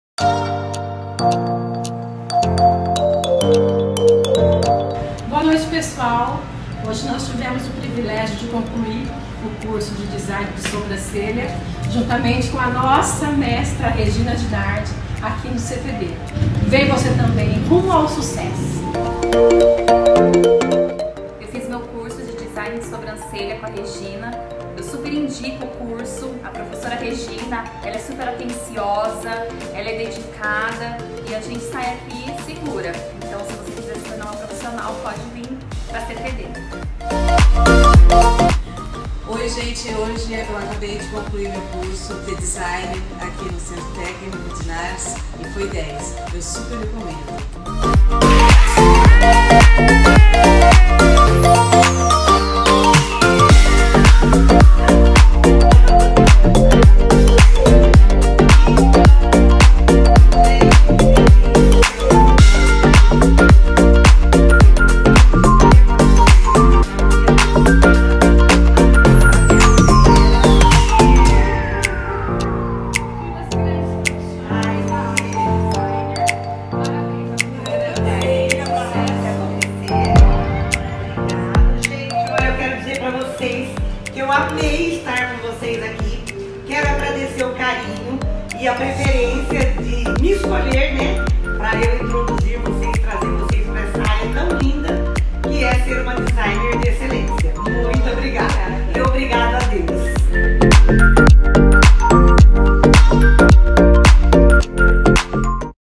Elas Realizaram um SONHO!! 🥰
FINAL DO CURSO DESIGNER DE SOBRANCELHAS!! 🥂
Ja caminhos com mil léguas de distância, mas todos começam com um primeiro passo!!
E o passo está dado!!
Quero parabenizar cada uma de vocês, pela dedicação, foco e persistência!!
Vocês foram maravilhosas!! 🥰
E quero agradecer a cada uma, pelo carinho e confiança, por me escolher para ensinar a vocês essa profissão tão linda, de embelezamento do olhar
O curso acabou, mas nossa ligação continuará sempre!! Pois vocês fazem parte da minha  história!! 🙏❤️
PARABÉNS 🎊 MENINAS!! 👏🏻👏🏻👏🏻👏🏻👏🏻👏🏻
. Quer saber mais ?? 🙋🏼‍♀️🙋‍♀️
Entre em Contato 👇
Centro_tecnico_dinards .
Rua Treze de Maio 17.15 Bauru/SP
Os Cursos mais completos do Brasil!!